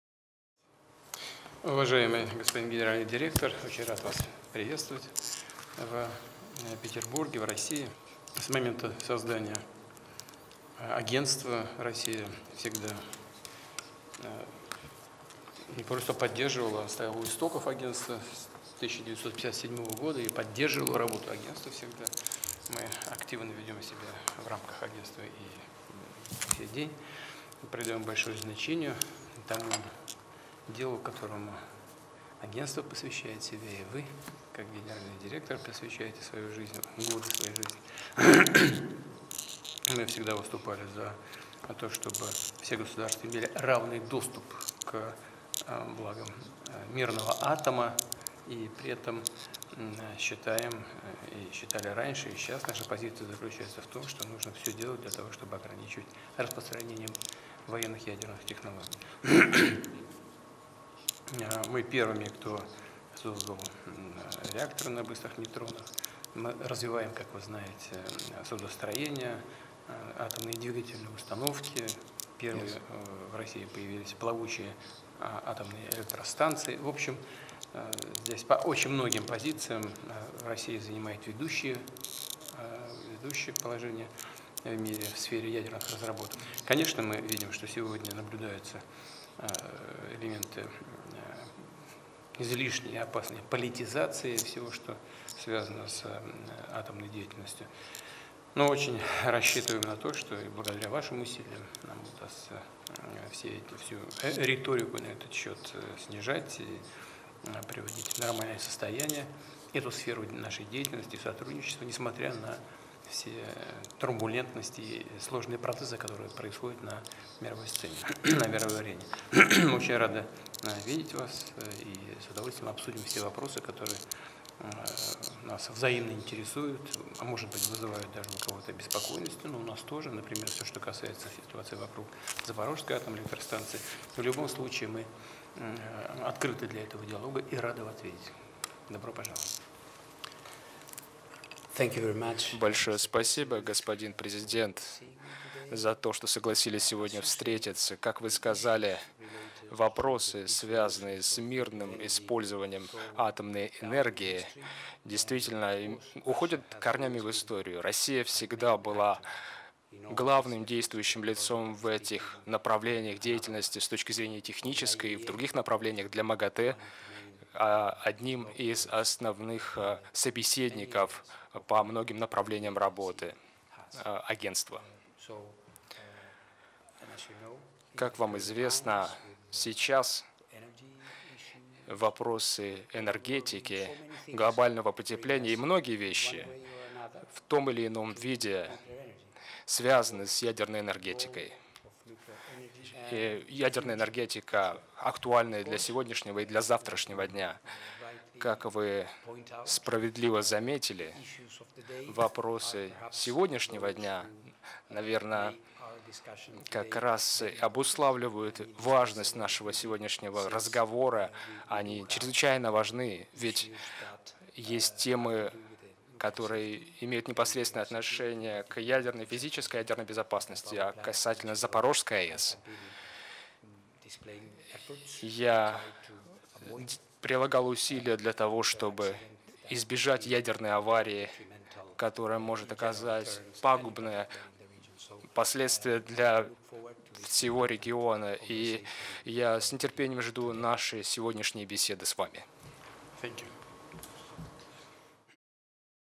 Встреча с генеральным директором МАГАТЭ Рафаэлем Гросси
Владимир Путин встретился с генеральным директором Международного агентства по атомной энергии (МАГАТЭ) Рафаэлем Гросси.